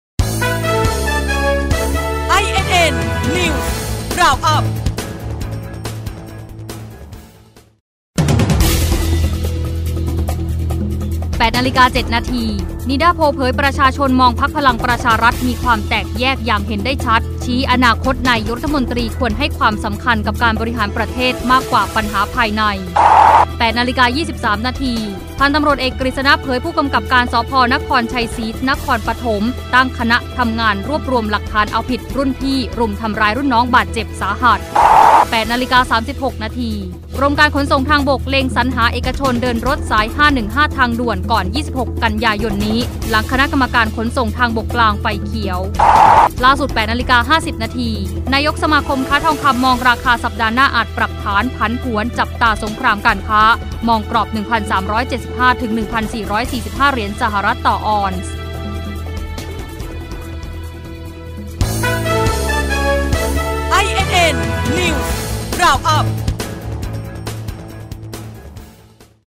เกาะสถานการณ์ 09.30 น.(07-07-62)
08.07 น.คนมองพปชร.แตกแยกภายในอย่างเห็นได้ชัด
08.23 น.ตั้งชุดคลี่คลายคดีรุ่นพี่รุมรุ่นน้องสาหัส
08.36 น.สรรหาเอกชนเดินรถสาย515ทางด่วน
08.50 น.สัปดาห์หน้าราคาทองผันผวน